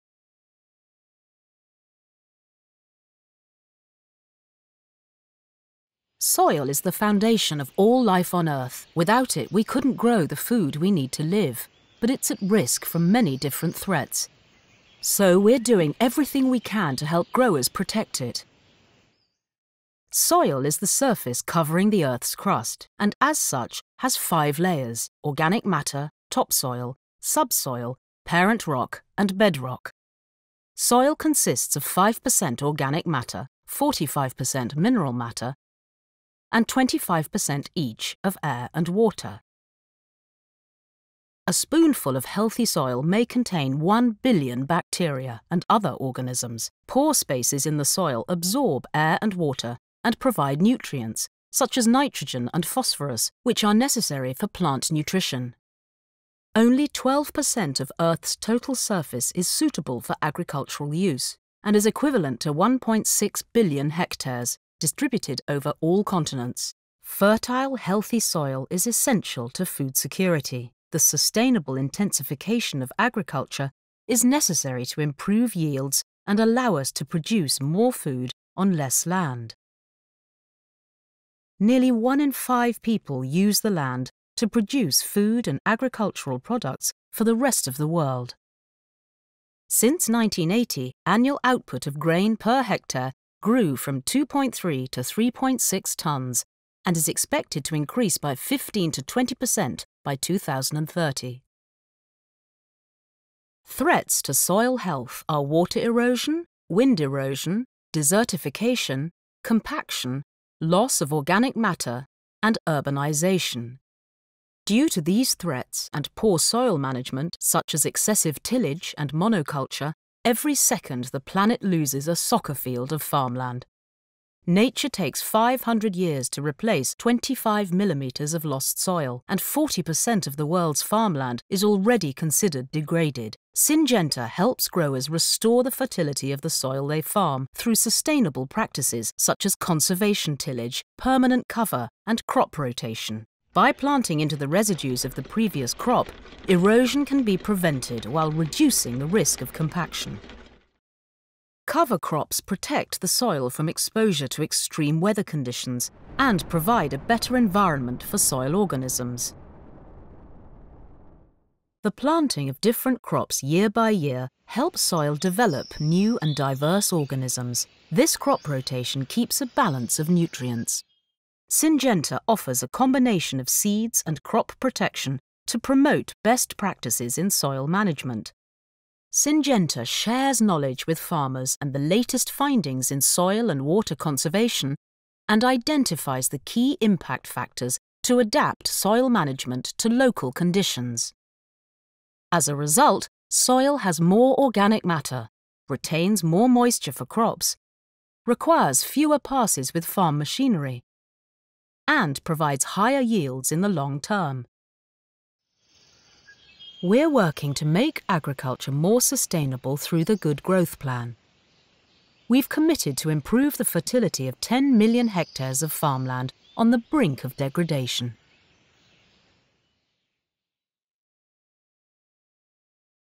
Why is soil so important?
Fertile, healthy soil is essential for agriculture and food security. Without it we couldn’t grow the food we need to live. But it’s at risk from many different threats: erosion, degradation, poor soil management, desertification and urbanization. Syngenta works with growers to help them restore the fertility of the soil they farm. We’ve committed to improve the fertility of 10 million hectares of farmland on the brink of degradation. to find out more.